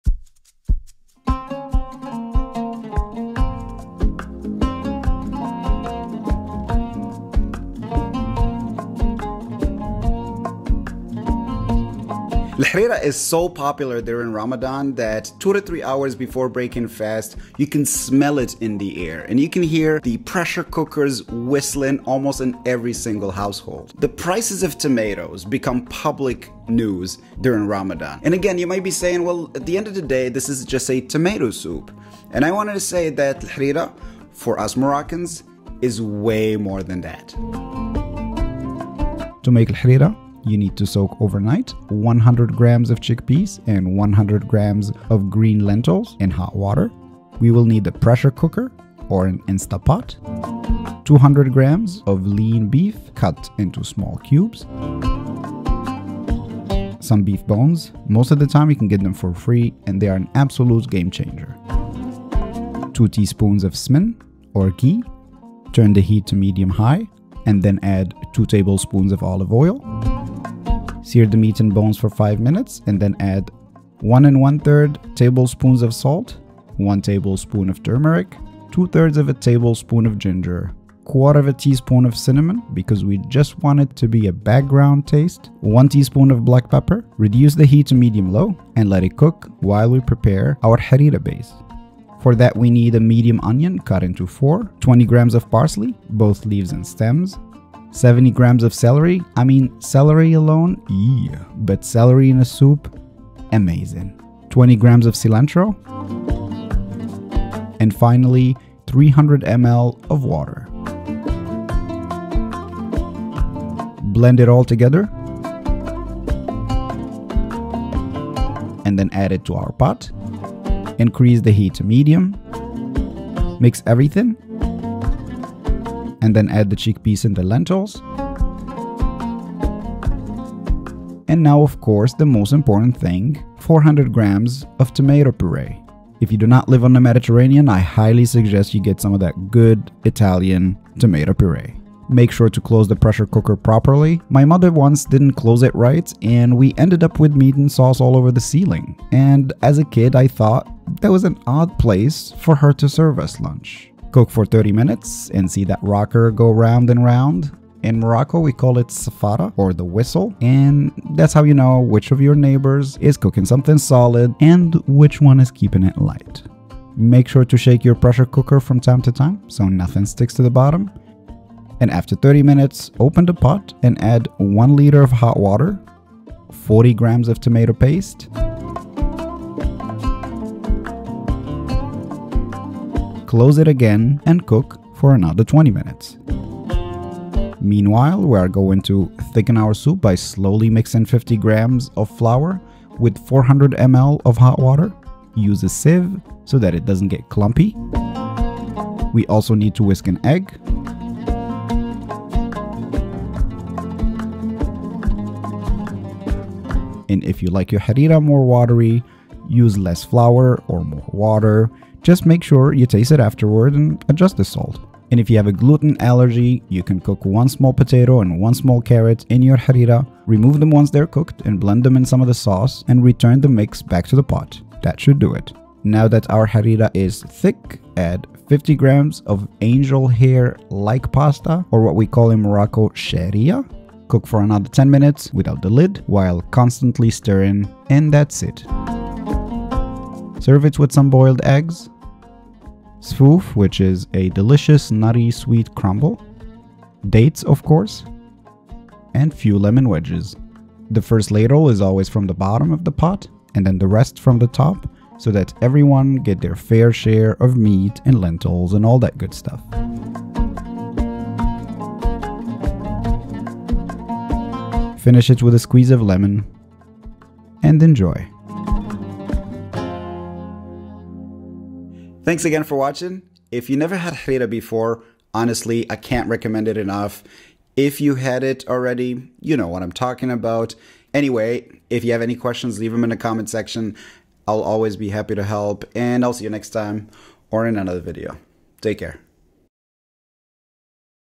How to make an authentic Moroccan Harira
Harira is more than just a soup. For most Moroccans, it is centre of the Iftar (Breaking fast) table. It has almost a cult status and it lives up to the hype. Harira is an amazing tomato soup. It’s a delicious mix of spices and fresh herbs, with vegetables (mainly tomato). You can cook it with or without meat but if you can, use bones and your Harira will be at whole other level !!!

Ingredients:
Beef - Cubed 200 g
Lentils 100 g
Chickpeas 100 g
Shaaria (Thin Pasta) 50 g
Smmen (Ghee) 2 tsp
Ginger 2/3 Tbsp
Egg 1
Flour 50 g
Tomato Puree 400 g
Onions 1 Medium
Parsley 20 g
Cilantro 20 g
Bones 3 Small
Celery  70 g
Water for base mix 300 ml
Tomato Paste 40 g
Water for flour mix 400 ml
Water Hot 1000 ml